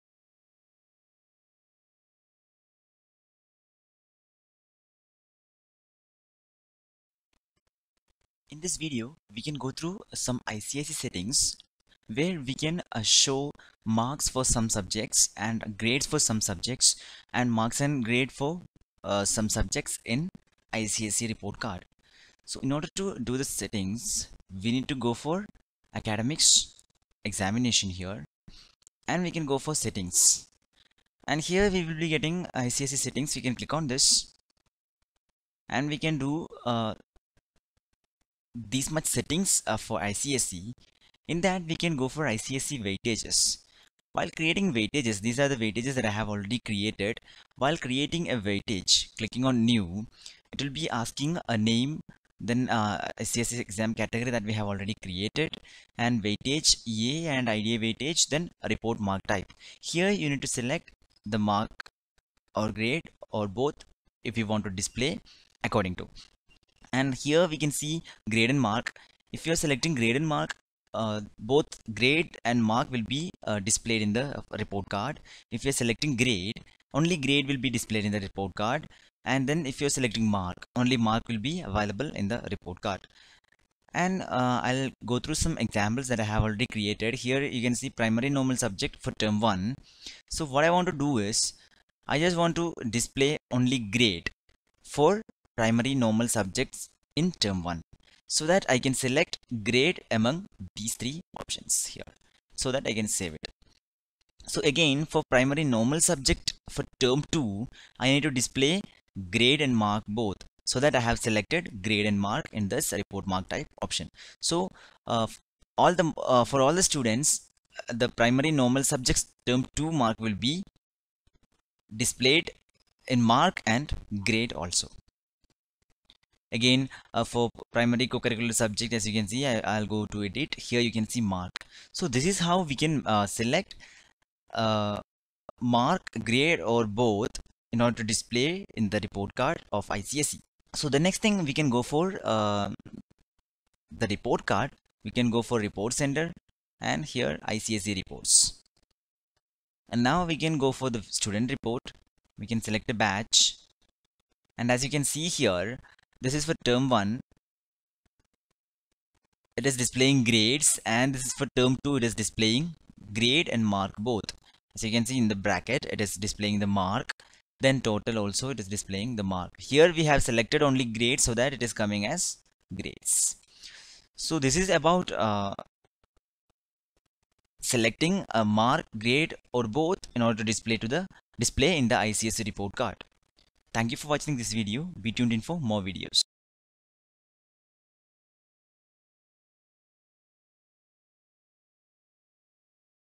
Configuring ICSE in order to show Mark/Grade/Both in the report card in Fedena
Watch this video to understand about configuring ICSE in order to show Mark/Grade/Both in the report card in Fedena